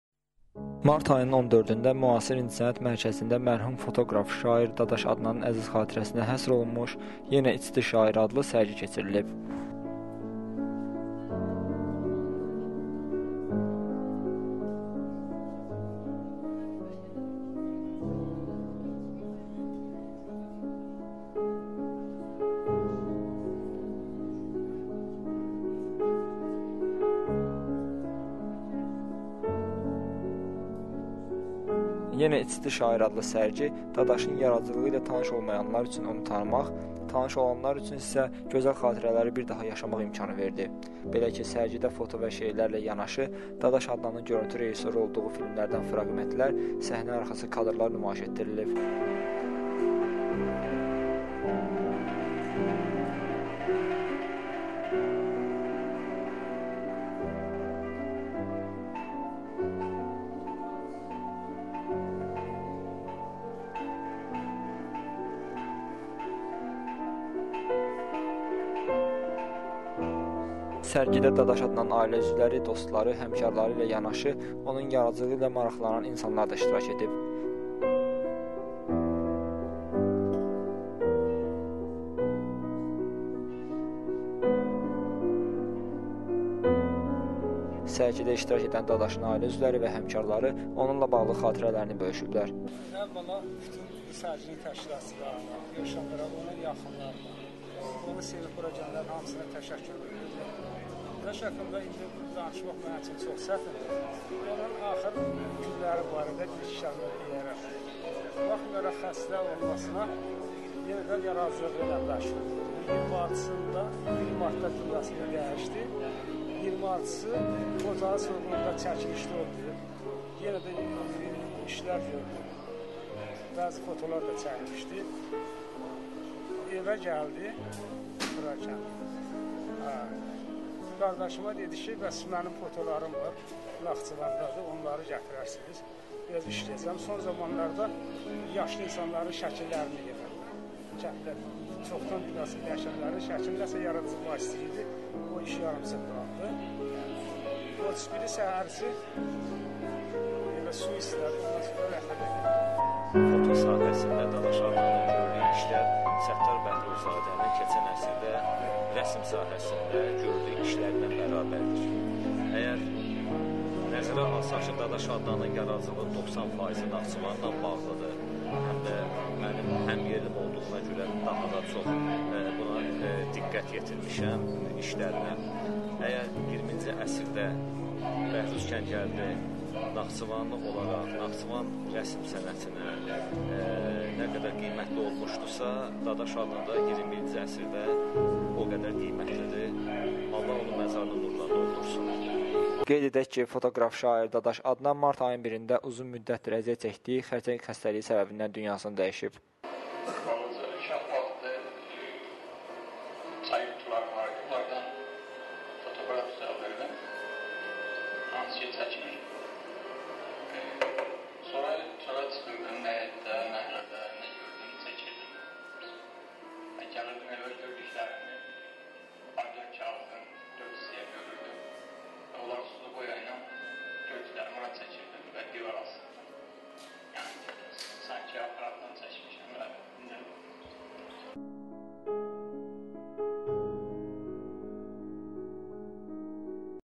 “Yenə içdi şair” – bir şeir... bir foto.. və bir rəsm haqqında əfsanə
14 mart 2015 ci ildə Müasir İncəsənət Mərkəzində mərhum fotoqraf Dadaş Adnanın çəkdiyi video və fotolardan ibarət sərgisi keçirilib.

“Yenə içdi şair” adlı sərgini Dadaş Adnanın həmkarları və yaxın dostları təşkil edib. Dostları deyir ki, mərhum fotoqraf sağlığında belə bir sərgi keçirmək arzusunda olsa da vaxtsız ölümü buna imkan verməyib.

“Biz onun arzusunu reallaşdırmağı qərara aldıq və bu sərgini təşkil etdik” - dostları belə deyir.Qeyd edək ki, Dadaş Adna bu il martın 1-də vəfat edib.

Tanınmış fotoqraflar, kinoşünaslar, incəsənət adamları və jurnalistlər də sərgiyə gəlmişdi. Bu da Dadaş Adnanın işlərinə böyük marağın olduğunu göstərir. Dadaş Adna (Adnayev Əbülqasım İsrafil oğlu) 1978-ci il, fevralın 3-də Naxçıvanın Sədərək rayonunda anadan olub. Hələ uşaq ikən şer yazmağa başlayan və kibrit qutularından fotoaparat düzəldərək “fotolar çəkən” Dadaş ixtiyarında olan kiçik imkanları belə dəyərləndirib böyük işləri araya-ərsəyə gətirirdi.

Dadaş ilk təhsilinə Sədərək rayon orta məktəbində başlayıb. Qarabağ müharibəsi başlayanda Sədərəkdə gedən müharibə üzündən 1993-cu ilin fevral ayında təhsilini davam etdirmək üçün Bakıya gəlir. Kimya-biologiya litseyinin 9-cu sinfinə qəbul olunur. Buranı bitirəndən sonra Qafqaz Universitetinə qəbul olunsa da ali təhsil almır... Əsgərliyə gedir, ancaq xəstəliyi ilə bağlı əsgərliyini də bitirə bilmir və Sədərəkə qayıdır. Orada futbol komandası yaradır, futbol oynayır. Daha sonra tale onu yenidən Bakıya gətirir. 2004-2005-ci illərdə AzTV-nin mədəniyyət xəbərləri departamentində və bir neçə prodakşn şirkətində operator kimi işləyir. Sonralar Qubanın Xəyal TV kanalına göndərilir. Qubada iki ay işlədikdən sonra səhhətinə görə oradan da çıxır. Onun həyatında depressiya dövrü başlayır. İki il Sədərəkdə yaşayır, küçə-küçə, məhlə-məhlə gəzərək fotoqrafiya ilə məşğul olur. 2007-ci ildə Regional TV kanalında operator kimi işə başlasa da sonradan arxiv müdiri vəzifəsinə təyin olur. Sonralar rejissor Rüfət Həsənov və Elvin Adıgözəlin filmlərinin görüntü rejissoru olur. “Dönüş”, “Dumanda”, “Buqələmun” kimi filmlərdə Adnanın adı var. Dadaşın bu soyadı indi daşın üzərinə yazılıb...